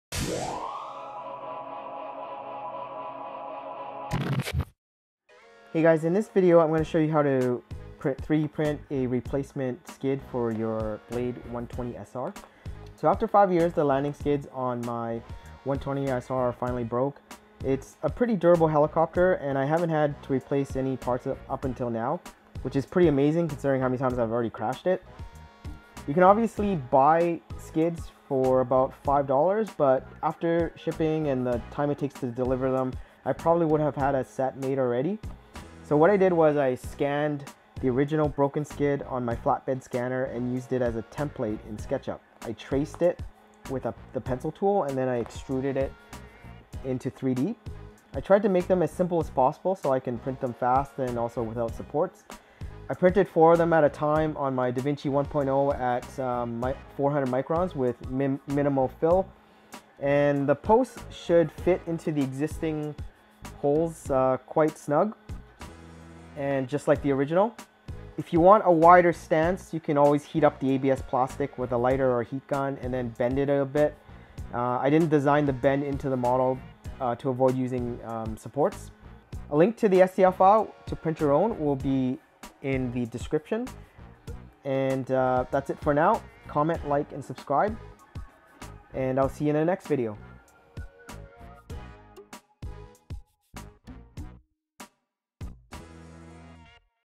Blade 120 SR Landing Skids 3D Print
3D printed landing skids for the E-Flite Blade 120 SR RC helicopter.

If you want a wider stance (recommended) you can heat up the ABS plastic with a lighter or heat-gun and then bend it a bit.  I didn't design the bend to avoid using supports.  A wider stance will help with stability so the 120SR doesn't tip over when landing or taking off.   Also stops it from spinning in place when taking off.